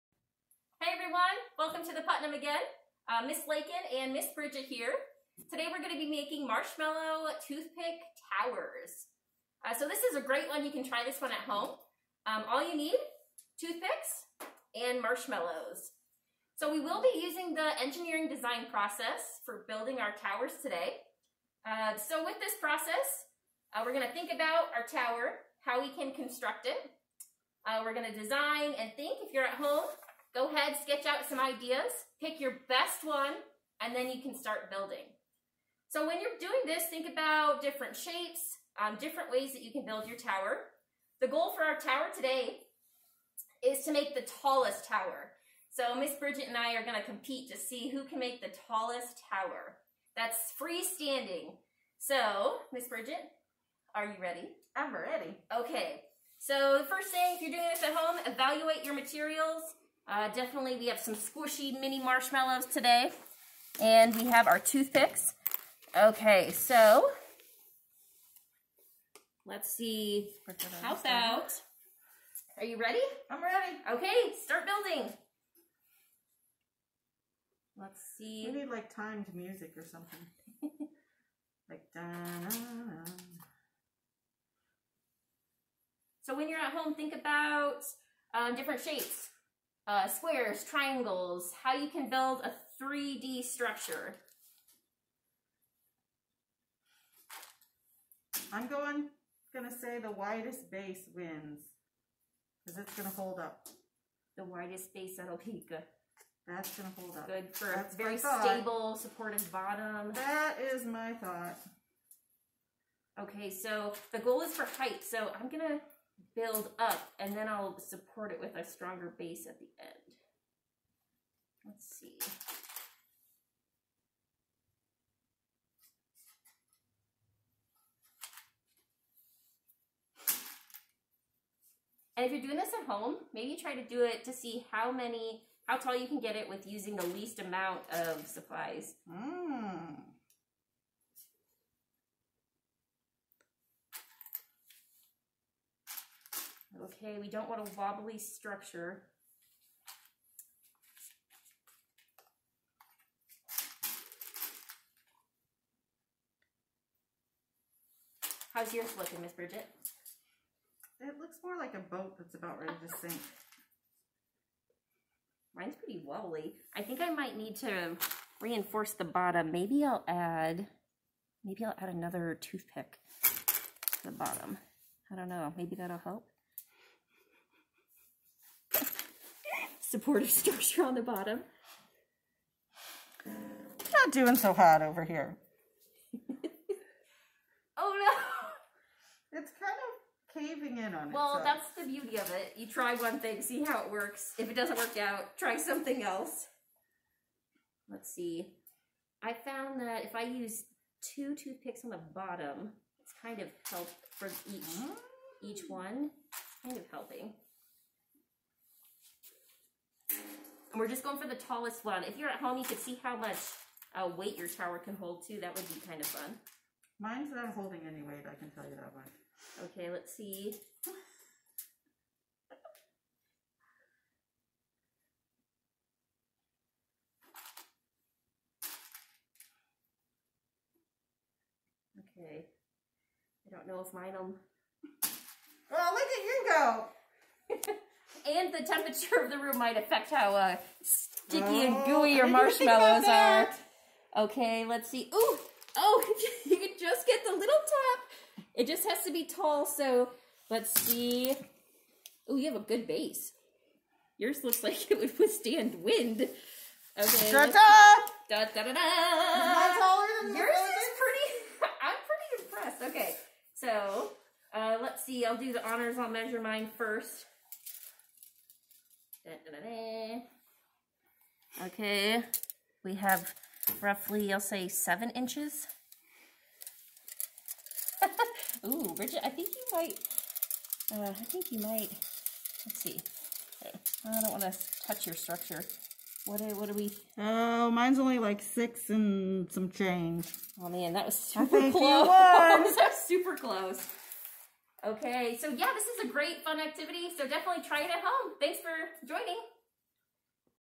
Marshmallow Tower
Who can build the tallest tower with just toothpicks and marshmallows?